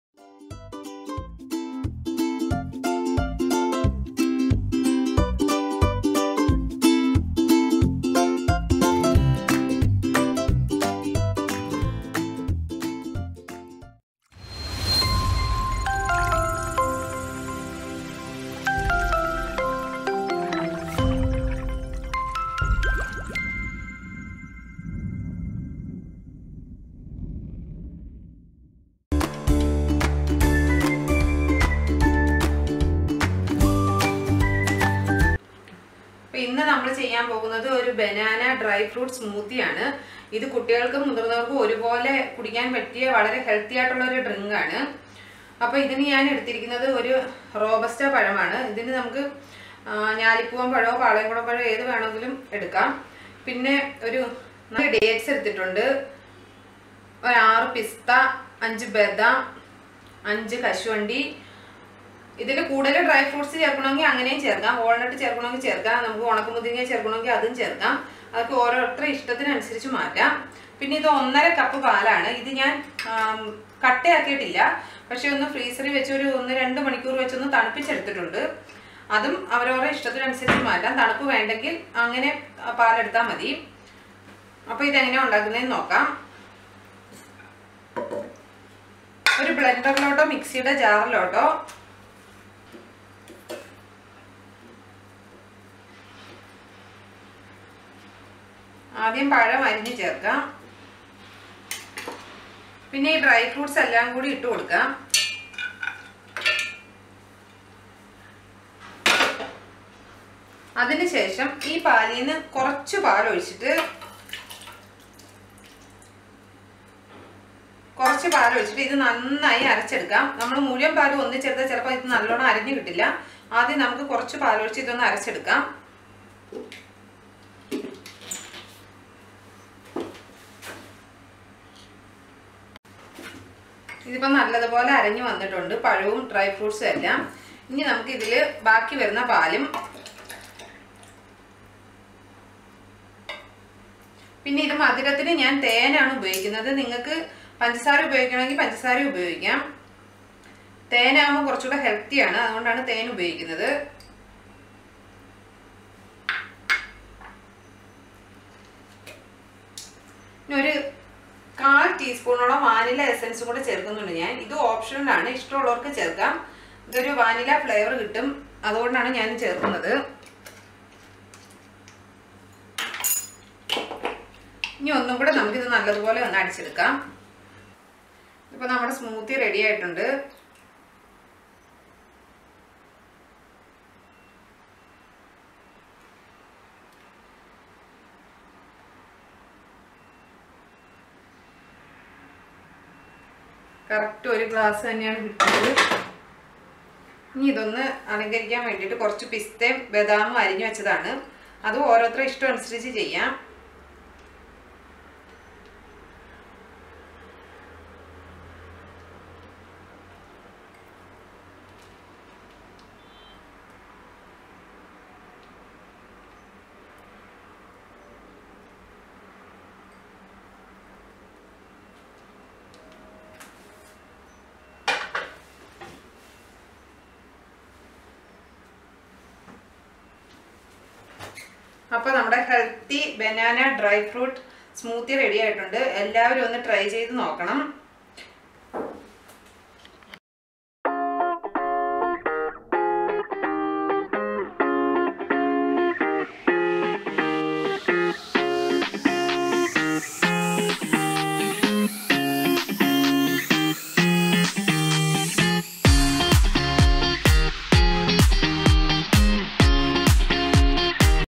വീട്ടിൽ രുചികരമായ Banana Dry Fruit Smoothie (ബനാന ഡ്രൈ ഫ്രൂട്ട് സ്മൂത്തി ) തയാറാക്കാം
Banana Dry Fruit Smoothie
(ബനാന ഡ്രൈ ഫ്രൂട്ട് സ്മൂത്തി )

Ingredients :
Banana
Chilled Milk - 1 1/2 Cup
Cashewnut, Badam
Pista, Dates
Honey, Vanilla Essence

Cut Banana into small pieces. Put all dry fruit to  mixer grinder and add little milk. Grind together smoothly and add balance milk. Add little vanilla essence.